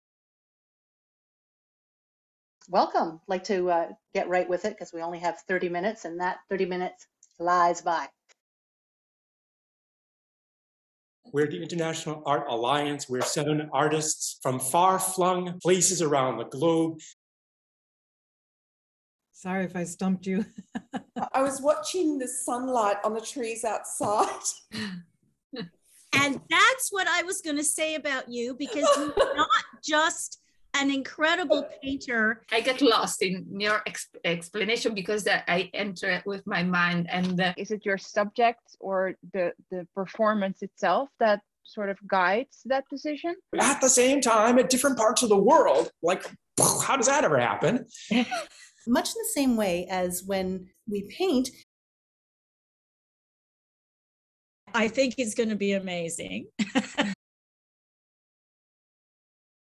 Palimpsest Promo 3
7 Artists, 3 continent, 1 epic project!
Join our live events ever Thursday at 3PM EST and witness the creation of 'Layers of Discovery'.

While the pandemic raged 7 artists from 4 different countries, formed a special virtual friendship and decided to embark on an epic art journey together.

By sending physical panels around the world they will create a series of original artworks. Each artist adding a layer in their own distinct medium and style, resulting in 98 layers divided between 14 paintings.

We have just started our test run and we would like to invite you to join us every Thursday at 3PM EST for a weekly live event where we will share our stories and experiences while we create this epic project together.